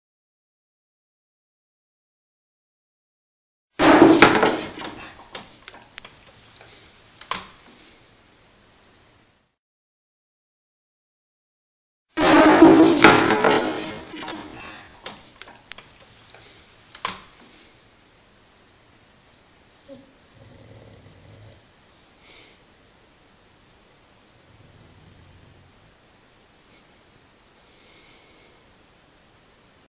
Put something in my bone
Prospect the Dog likes his rawhide bones with a treat shoved in the end -- meat, peanut butter, chewy dog treat. JUST PUT SOMETHING IN HIS BONE.